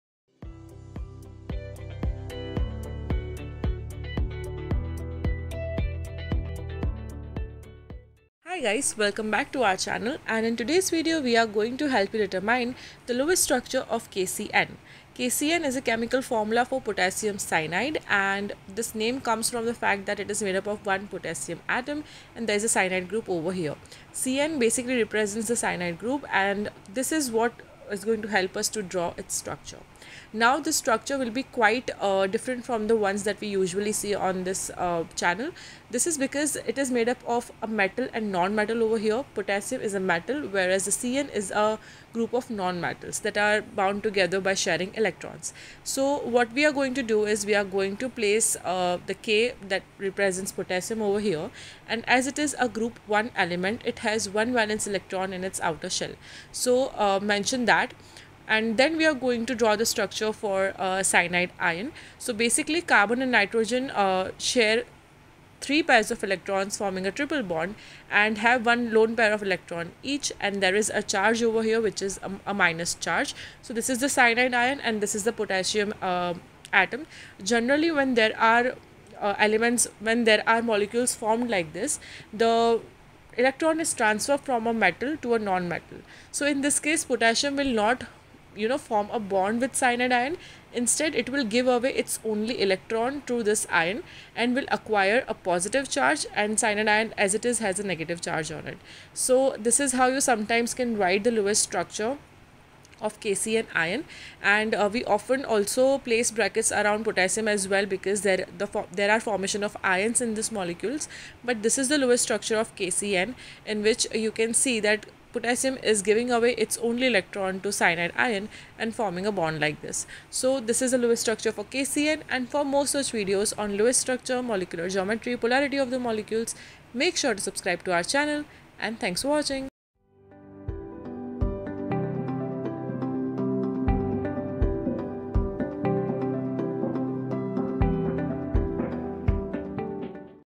KCN Lewis Structure (Potassium Cyanide)
Hi Guys!

Today in this video we are going to help you determine the Lewis Structure of KCN by using a step-by-step approach. We first find out what the molecule consists of and then try to determine its structure using the data we have and periodic tabe.

To find out the Lewis Structure of Potassium Cyanide, keep watching!

In this video we are going to look at the total number of valence electrons, bonds formed between the atoms and the formal charges on individual atoms.